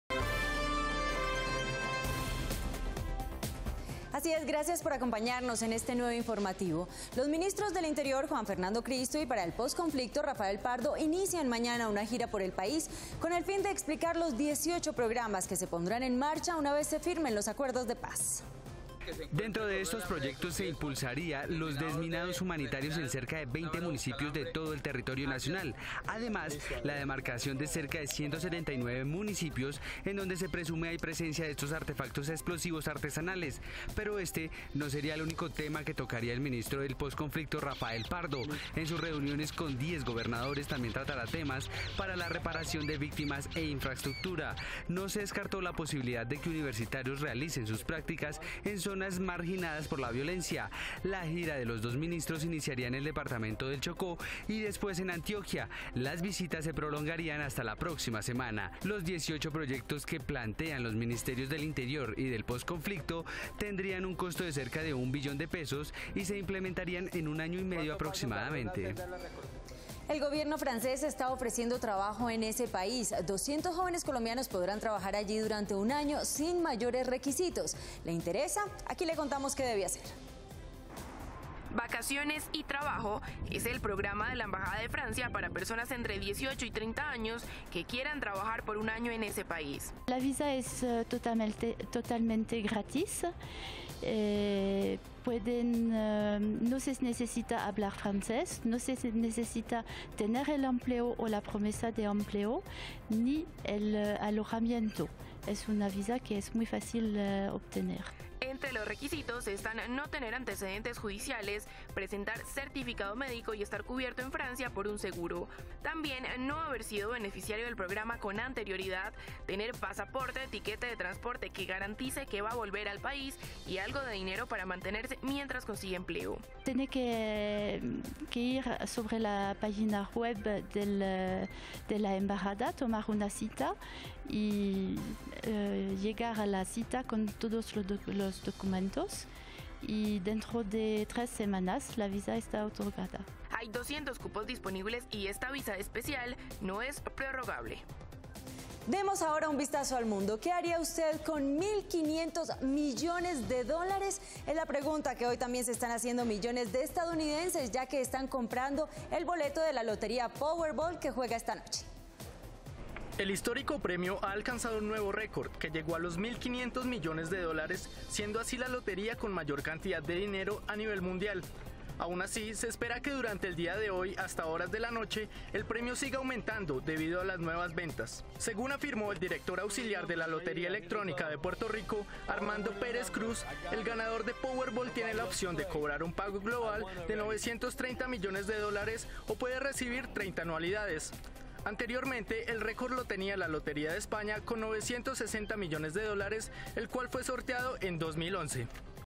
Ministros harán gira informativa sobre proceso de paz
Los ministros del Interior, Juan Fernando Cristo, y para el Posconflicto, Rafael Pardo, inician mañana una gira por el país, con el fin de explicar los 18 programas que se pondrán en marcha una vez se firmen los acuerdos de paz.